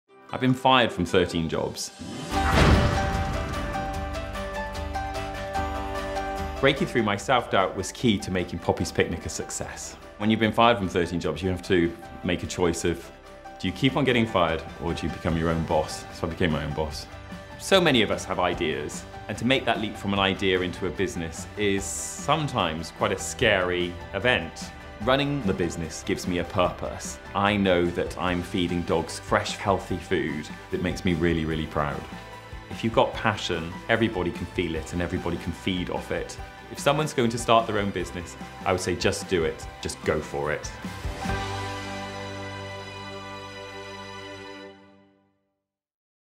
Rethinking Business with Dylan Watkins - NatWest Business
Self-doubt shouldn't stand in the way of your success. Dylan Watkins didn't fit into corporate life, but he found his passion in creating Poppy's Picnic, a raw dog food company.